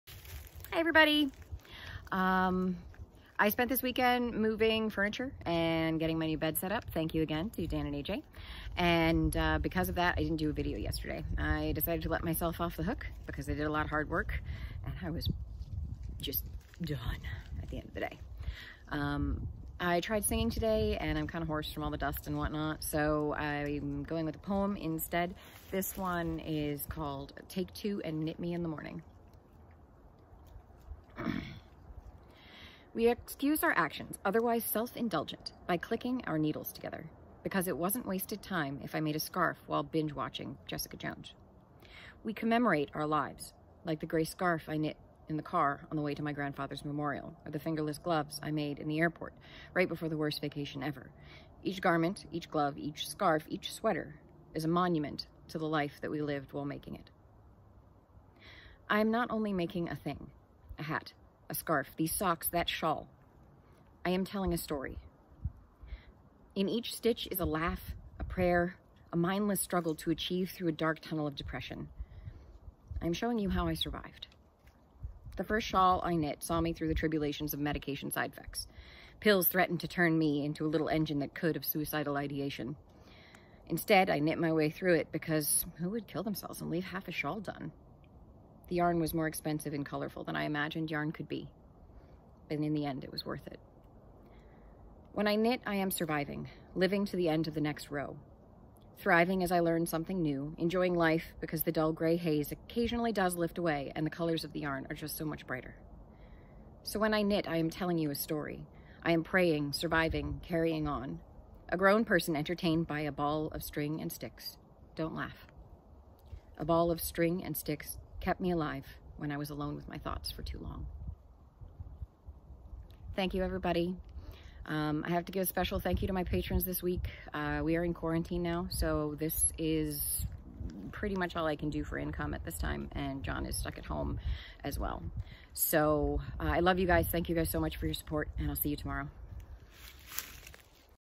Take Two and Knit Me in the Morning - Poem - Distance Bardic!
Thank you to all my patrons! Please enjoy this poem I’ve written and the sunny day around me!